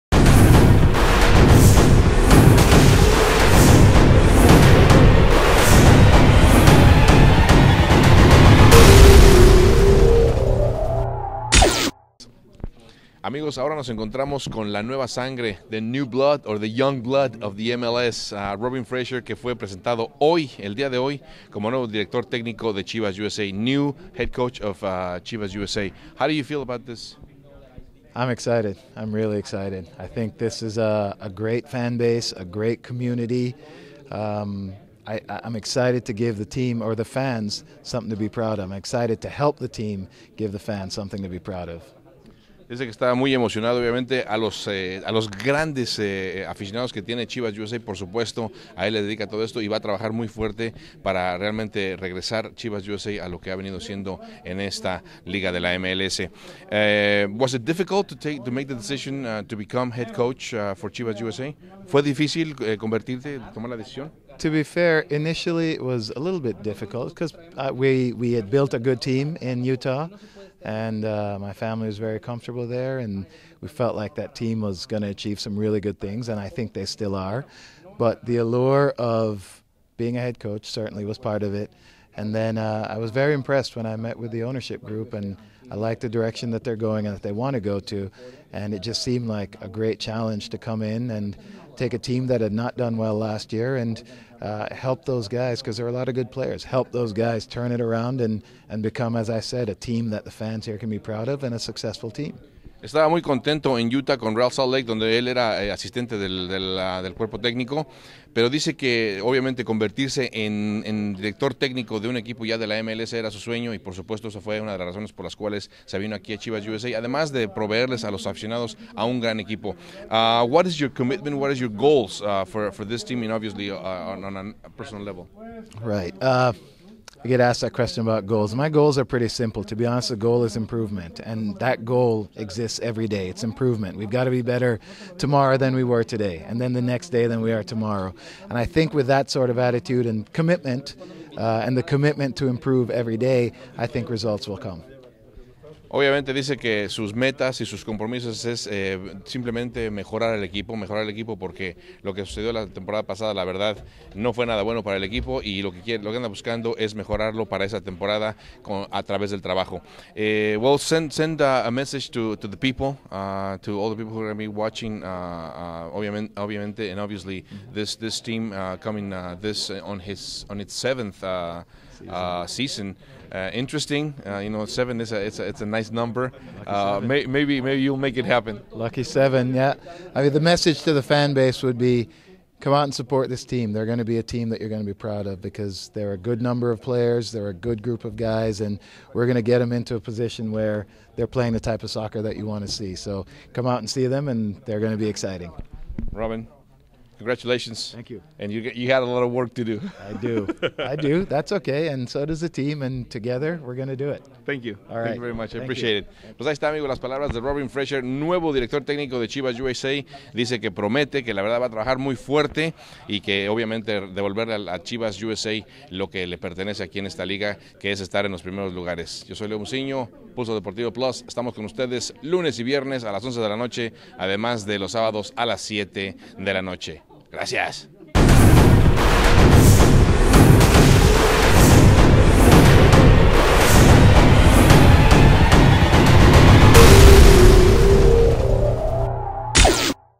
Entrevista con Robin Fraser , Nuevo Entrenador de Chivas USA en conferencia de prensa
Entrevista con Robin Fraser Nuevo entrenador de Chivas USA, en conferencia de prensa nos comenta sus planes con el equipo y sus metas a corto y largo plazo veamos.